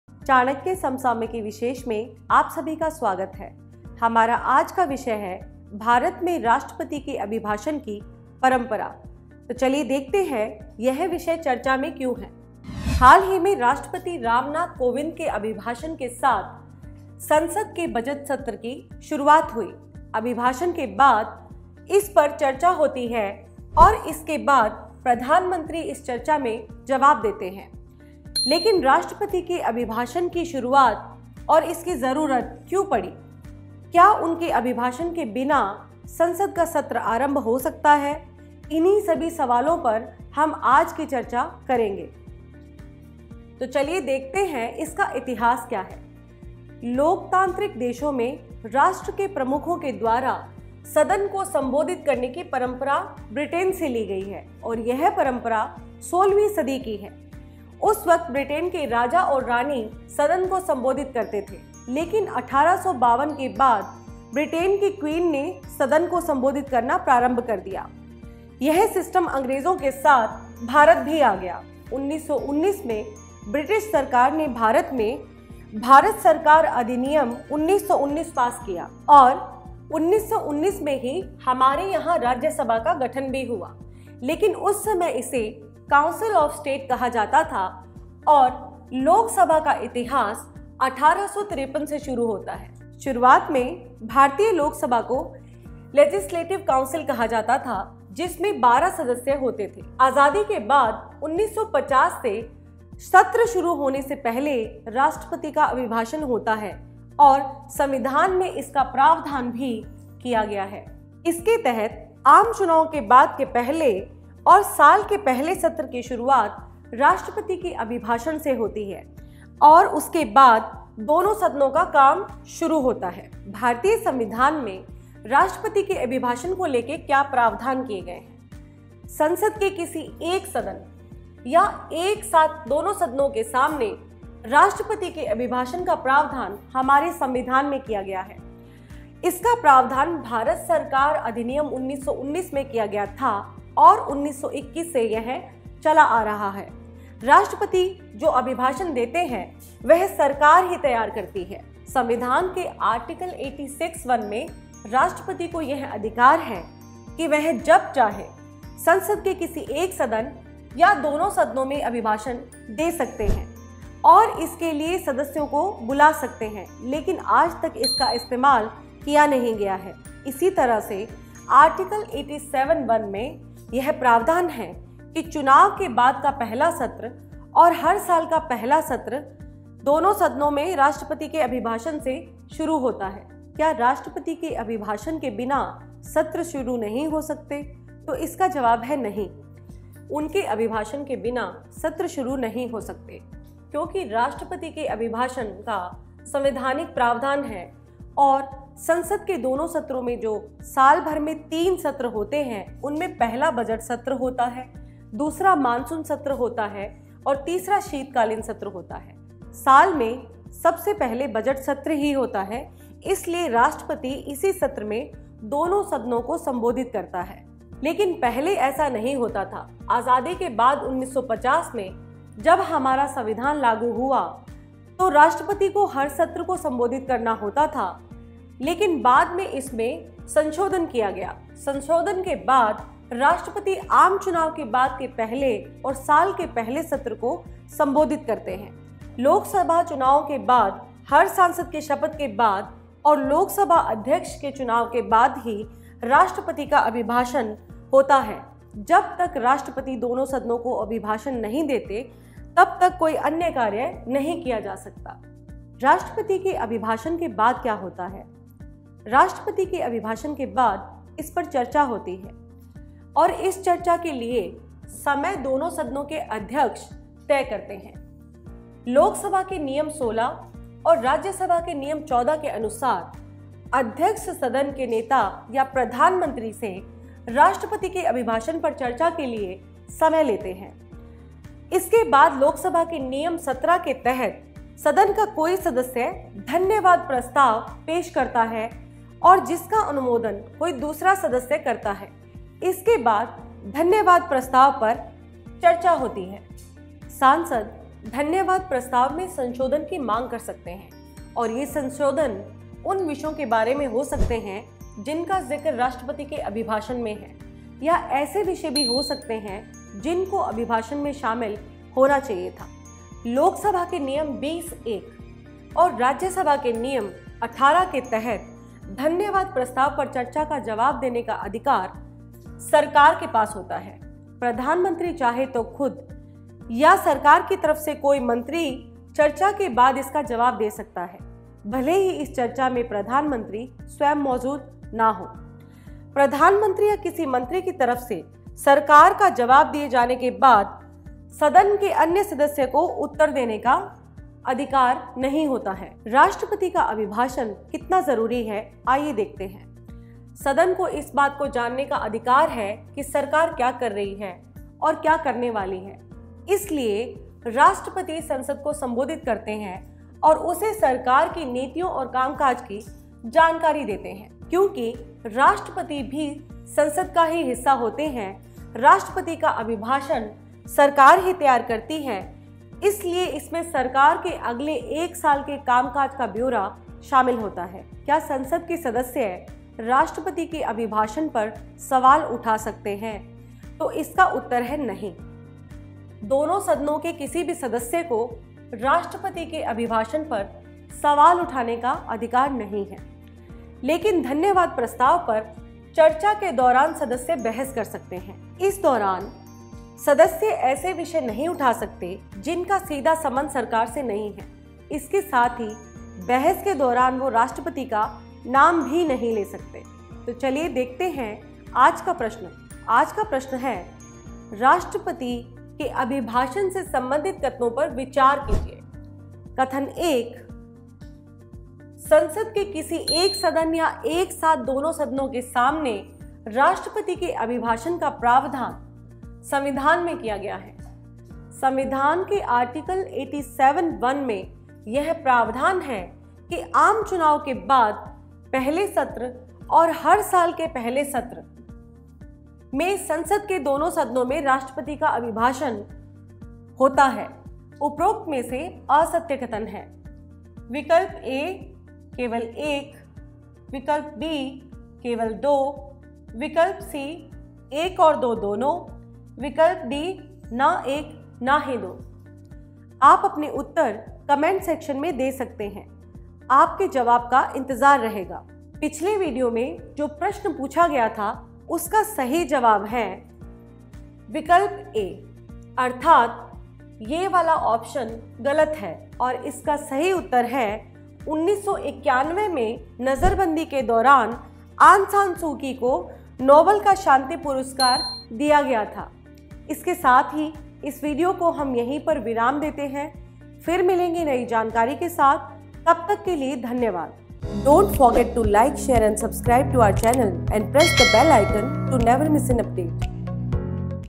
Importance Of President's Speech In India | Current Affairs Today | 5 February 2021 | Chanakya IAS
Daily Current Affairs Today's Topic is Importance Of President's Speech In India. The President's speech essentially highlights the government's policy priorities and plans for the upcoming year. The President may address both Houses of Parliament assembled together or either House of Parliament separately.